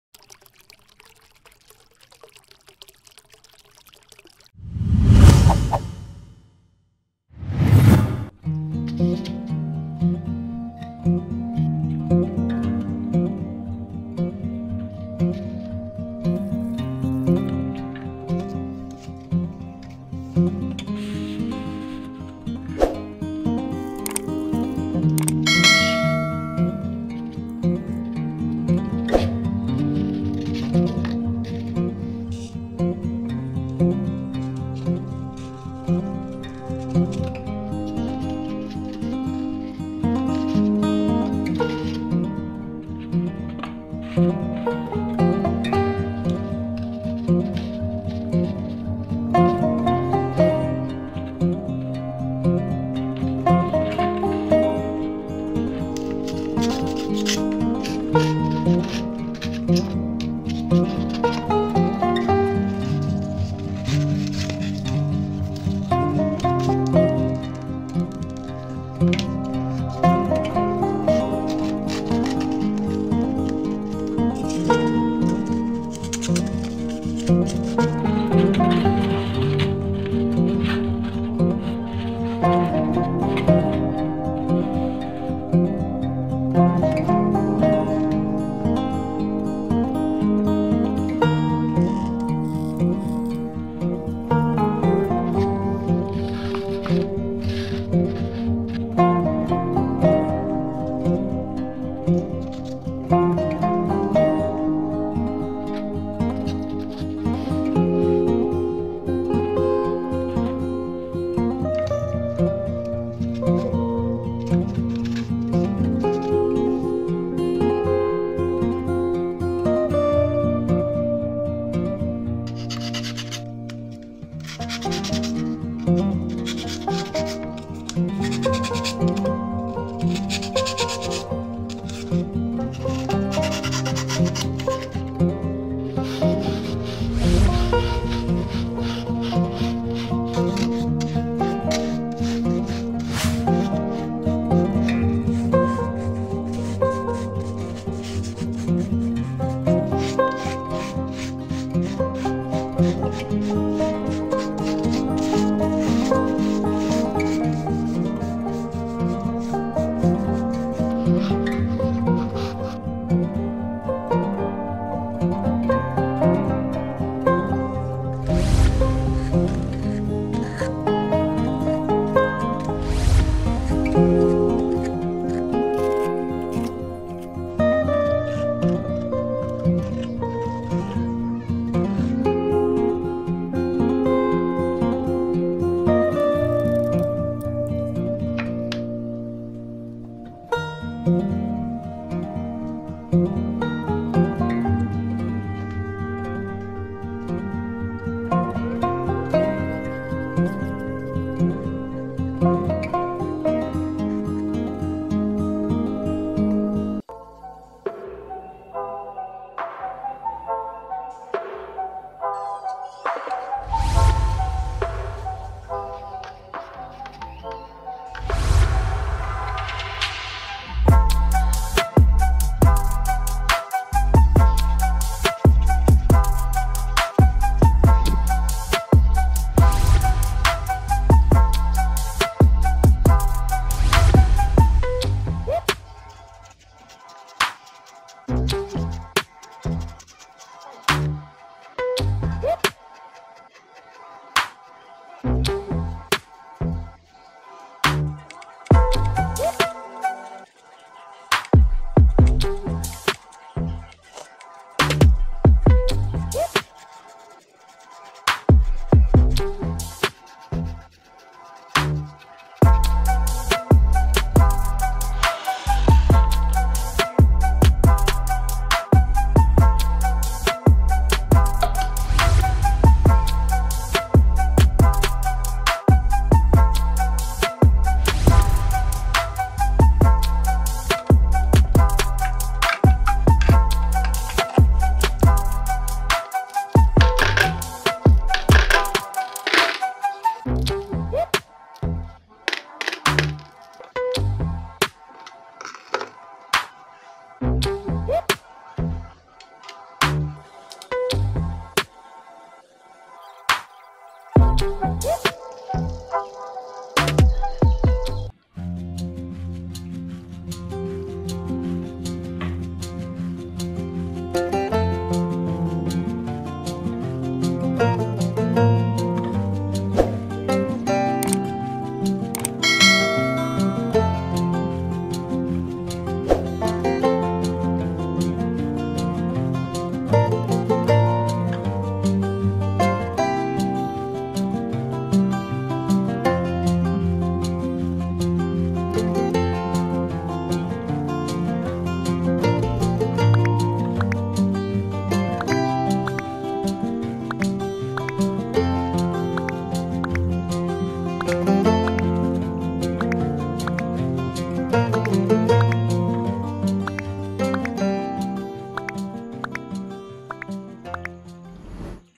Homemade mini water fountain styrofoam cement diy waterfall
Please give us a like :D
In this video, I'll show you how to make an amazing DIY water fountain by STYROFOAM. The fountain base was made with white STYROFOAM and the body was made using cement and water. you can use this as a mini-waterfall aquarium on your tabletop. also, this will increase your air cooling.